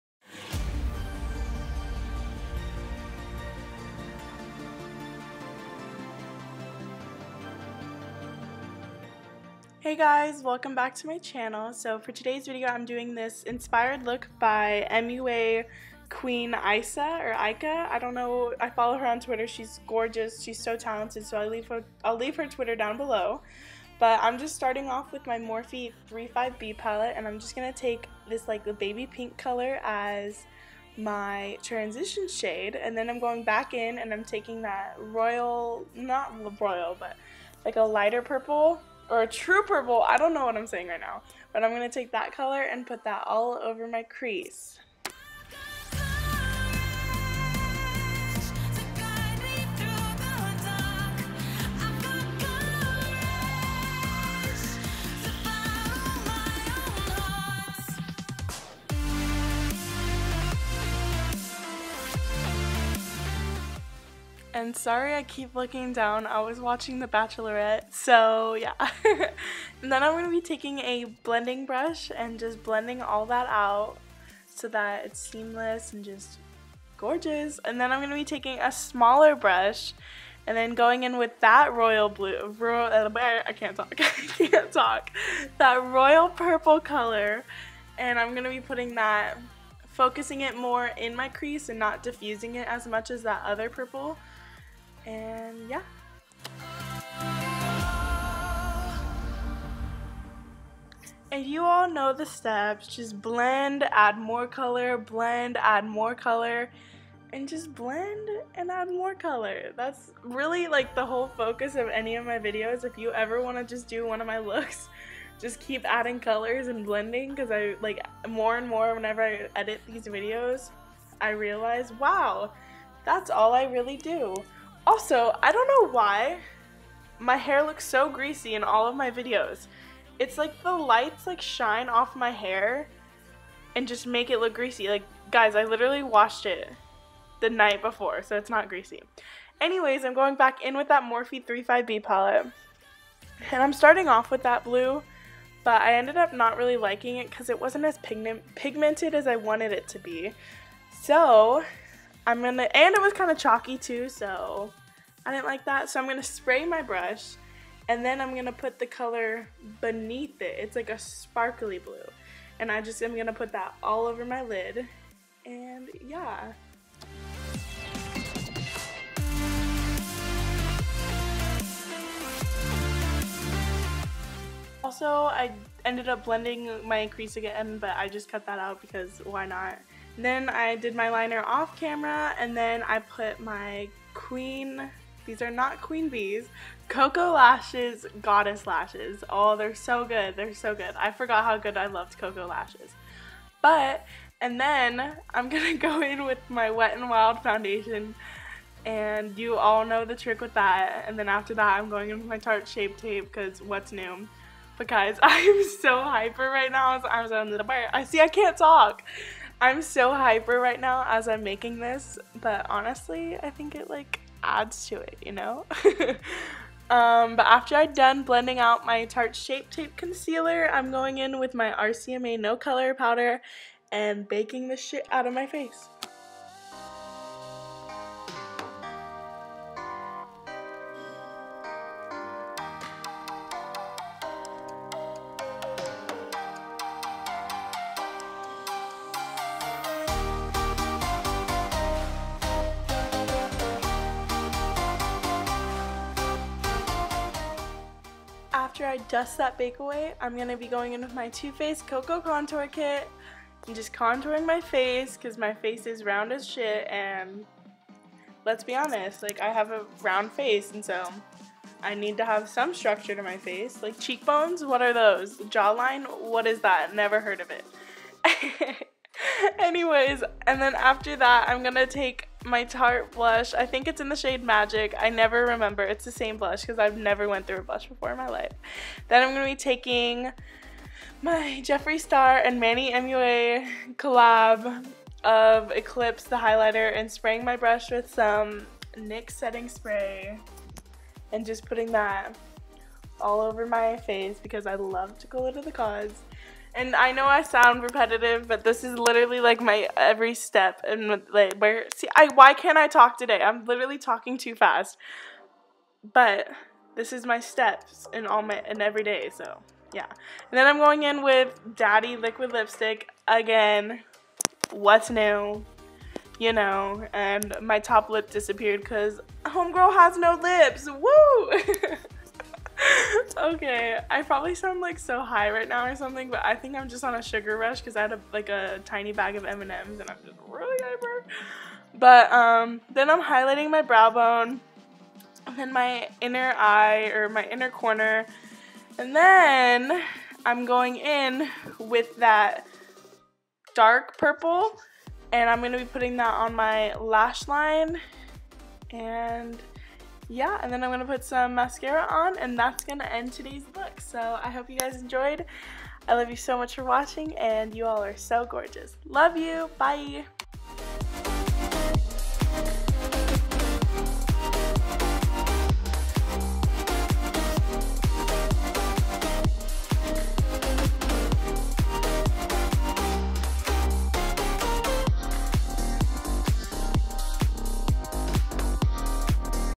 Cotton Candy Dream |Shelby Sellers
Hey guys! I got inspo for this look from @muaqueenica on twitter! Love you guys so much!